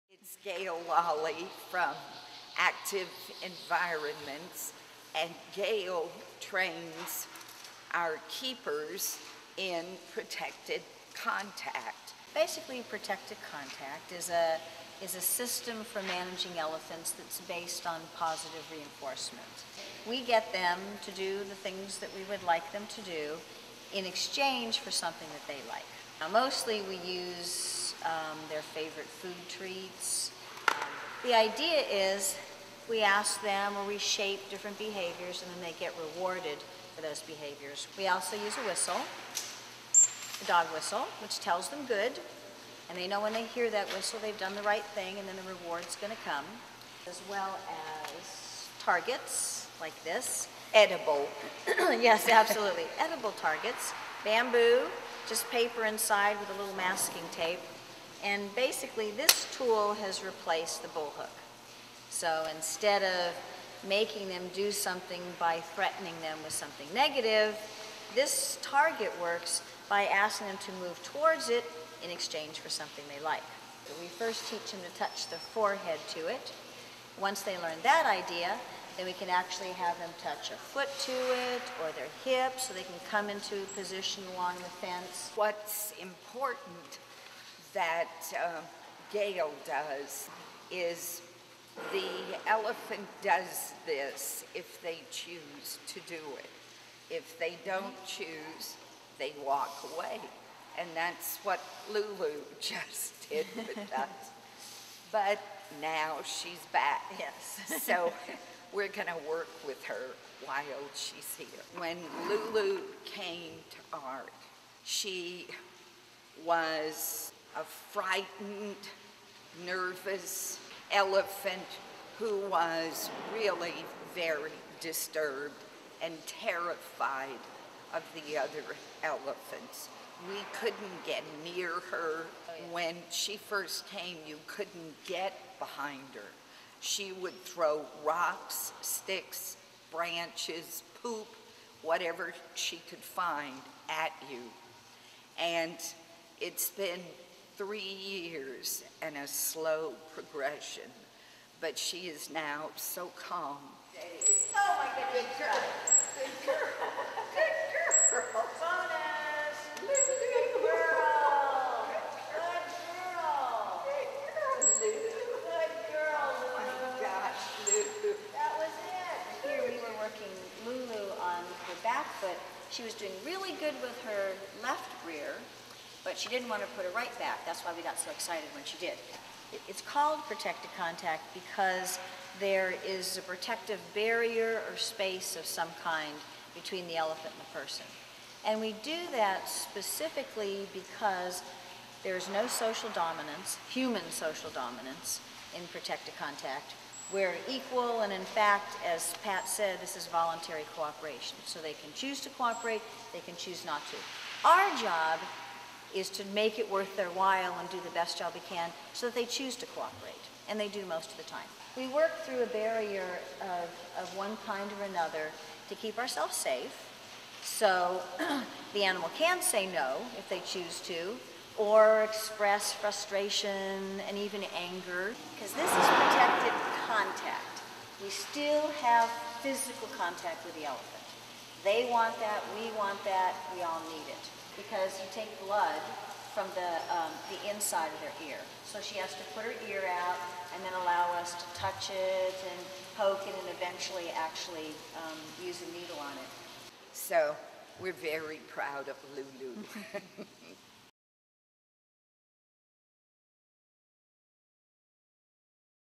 Elephant Protected Contact Training
Gail Laule, from Active Environments, uses protected contact to work with Lulu, an African elephant at the PAWS ARK 2000 sanctuary.  Protected contact is a method of managing elephants using voluntary cooperation and positive reinforcement to provide the care they need in captivity.

Join PAWS on Facebook!
Search — Performing Animal Welfare Society - PAWS